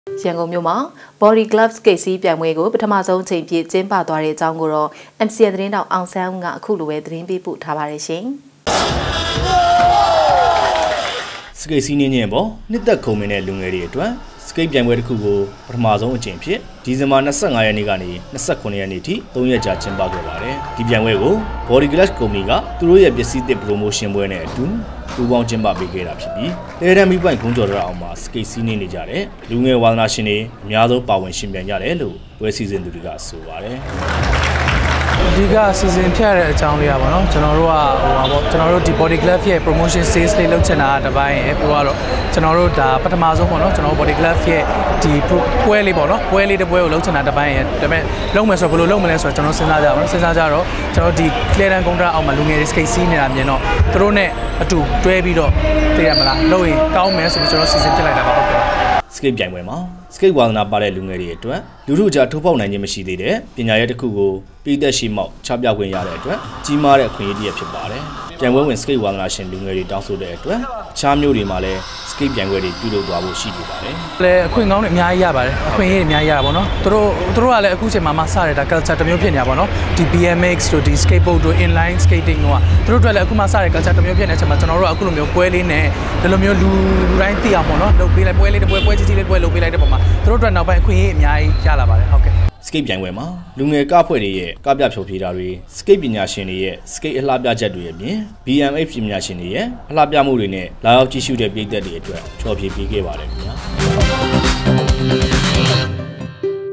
ရန္ကုန္ျမိဳ႕မွာ Body Glove စကိတ္စီးျပိဳင္ပြဲကို ပထမဆံုးအၾကိမ္အျဖစ္ က်င္းပ
ရန္ကုန္ျမိဳ႕မွာ Body Glove စကိတ္စီးျပိဳင္ပြဲကို ပထမဆံုးအၾကိမ္အျဖစ္ က်င္းပသြားတဲ့အေၾကာင္းကို MCN သတင္းေထာက္ ေအာင္ဆန္းဦးက တင္ဆက္ေပးထားပါတယ္။